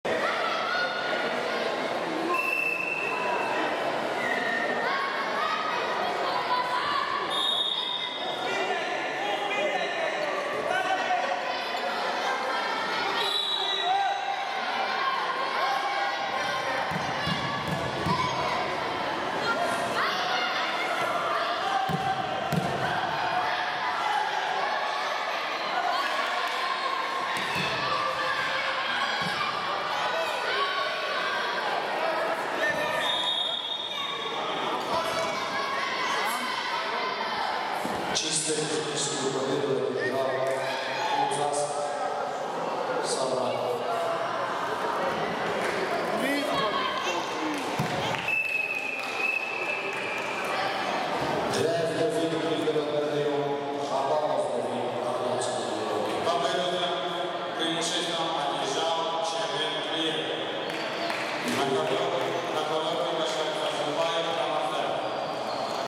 Плиев Чермен (Кировский р-н.) - Кабисов Ибрагим (Брут)
1/16 до 32 кг.
Республиканский турнир по вольной борьбе среди юношей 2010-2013 г.р., посвящённый Дню защитника Отечества.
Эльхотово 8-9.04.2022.